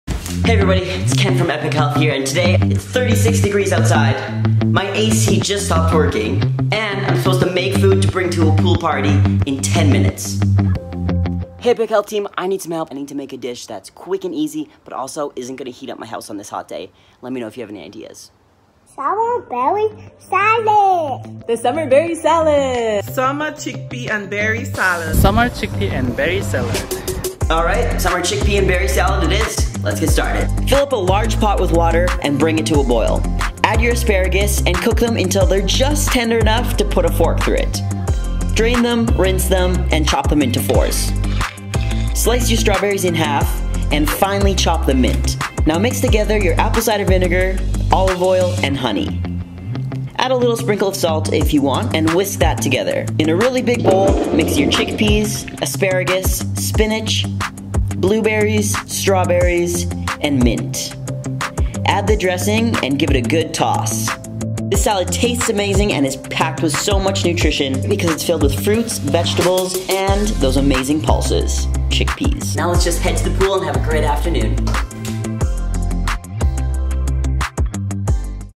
SUMMER CHICKPEA & BERRY SALAD RECIPE
How to use chickpeas in a summer salad that's nutritious and delicious!

Video created by: Ken Mullock, RD

Special thanks to the Saskatchewan Health Research Foundation for funding this project.